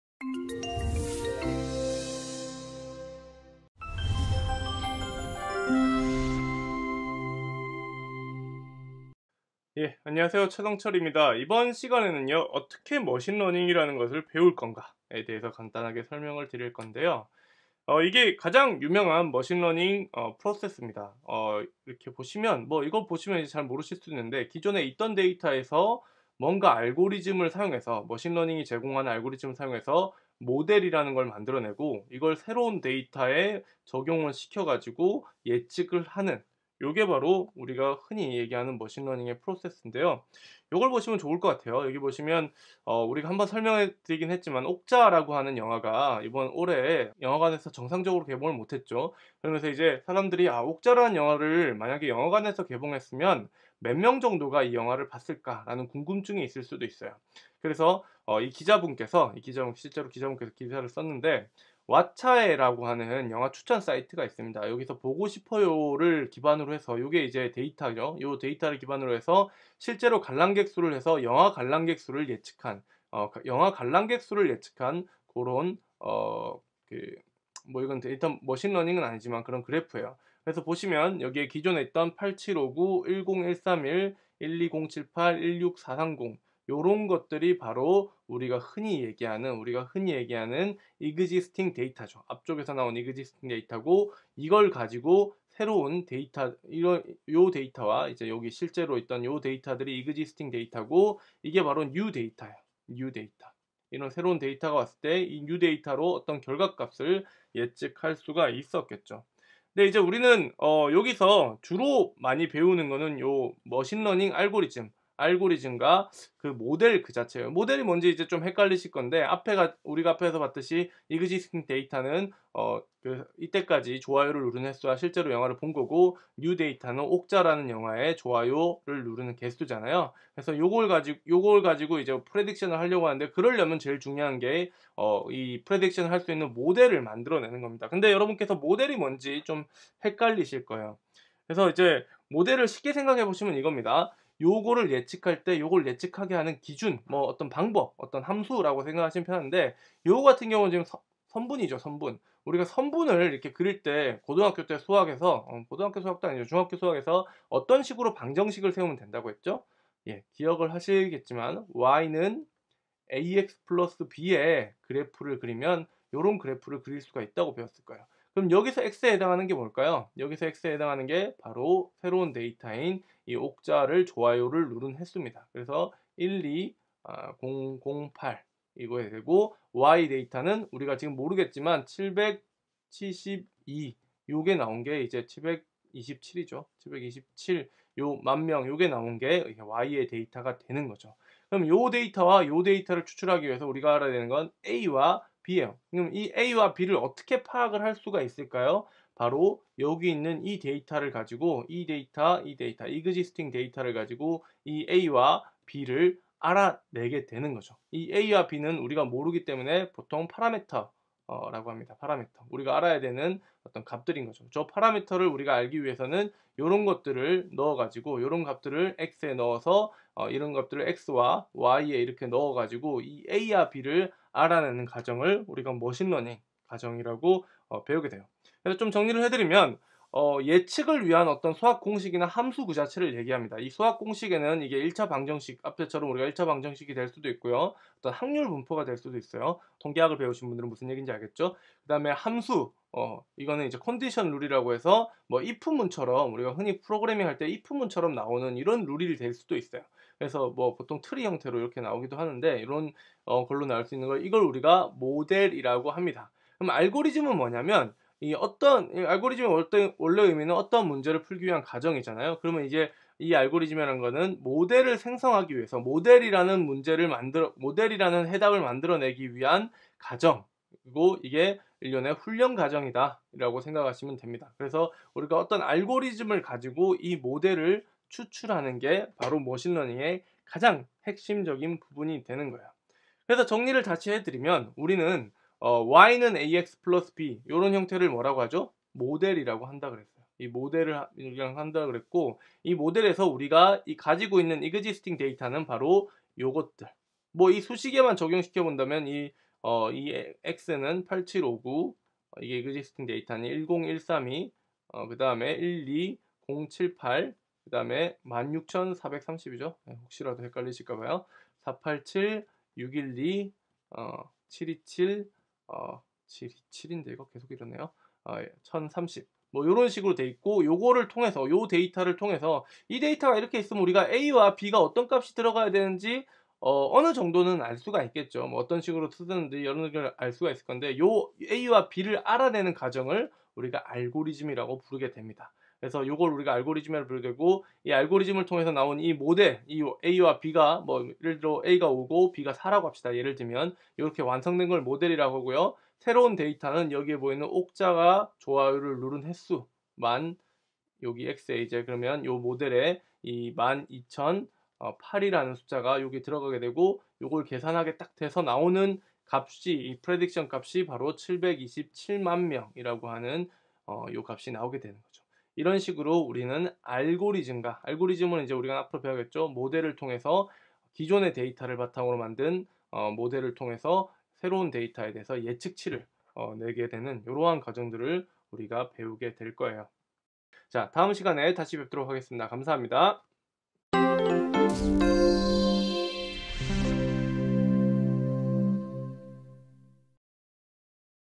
머신러닝 입문 강좌 | TEAMLAB X Inflearn | 1-3 How to learn machine learning
Machine Learning from Scratch with Python
본 강의는 TEAMLAB과 Inflearn이 함께 구축한 데이터 사이언스 과정의 두 번째 강의인 밑바닥 부터 시작하는 머신러닝 입문 입니다. 밑바닥부터 시작하는 머신러닝 입문은 Part I과 Part II로 구성되어 있습니다.